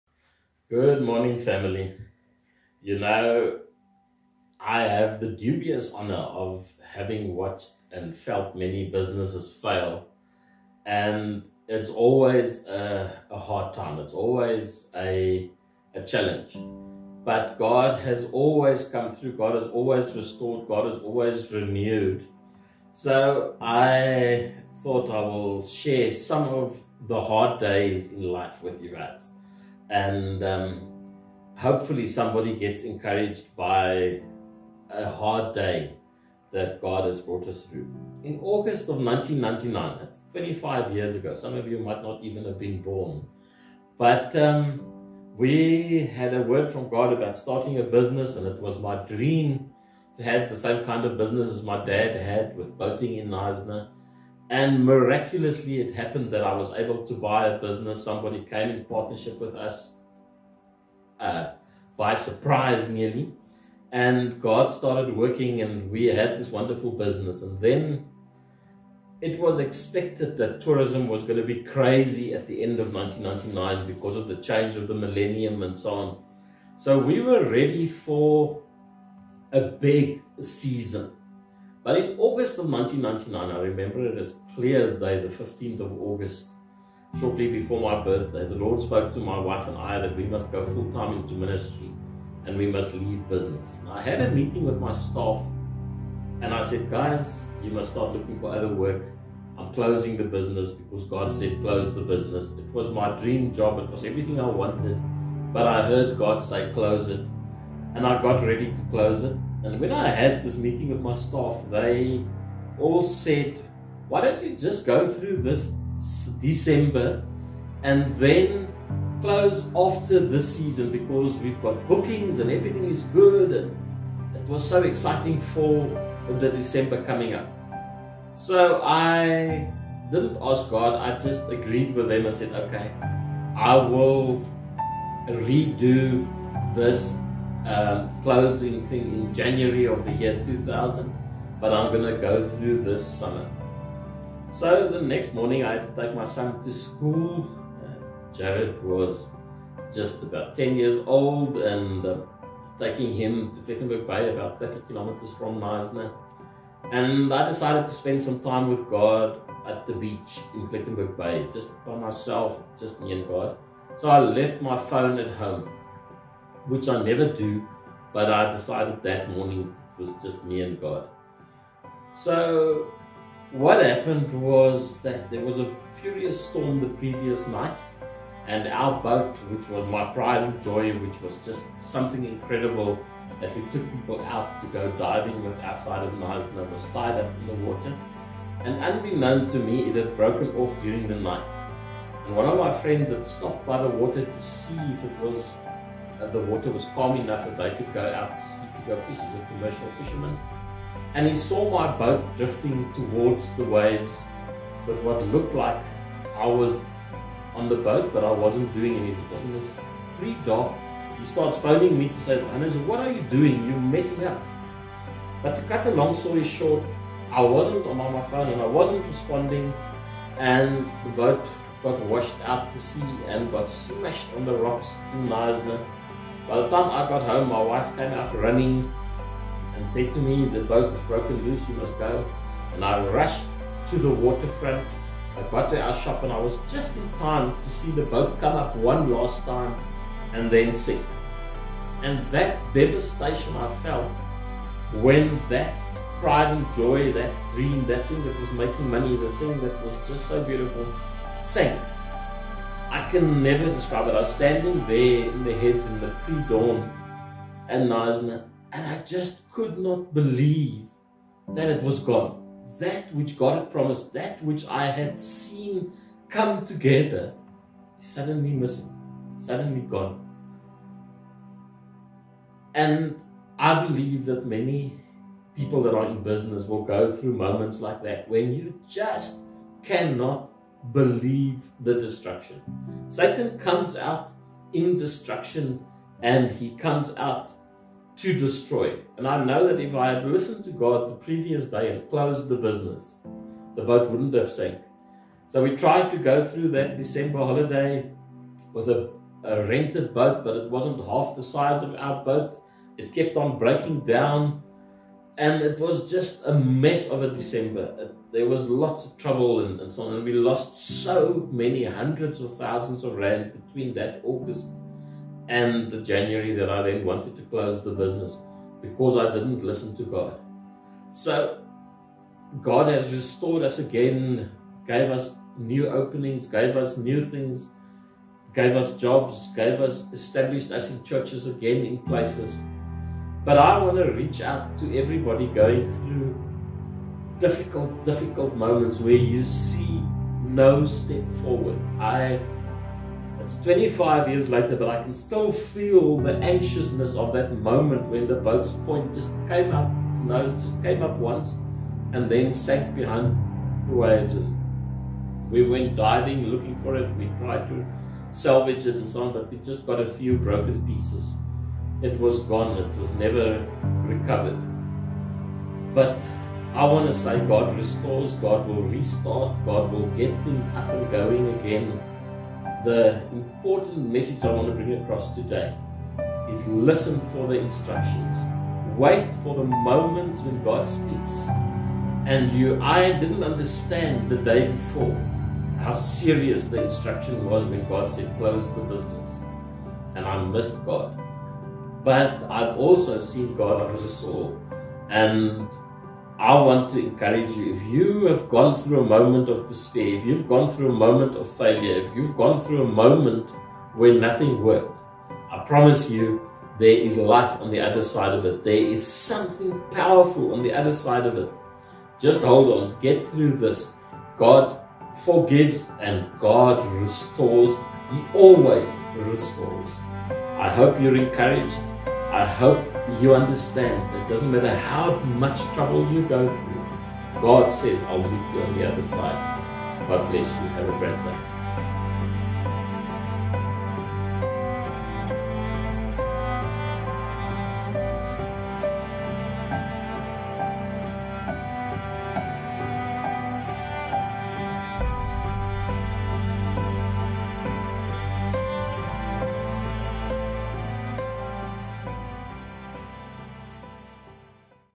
A Lifetime Story Chapter 1 // God Always Restores // Hannes Wessels
Apostle Hannes and Prophet Mandy Wessels have experienced the loss of business more than once. In this lifetime story, Apostle Hannes shares candidly about the first of such seasons and the resulting restoration when we humble ourselves before God.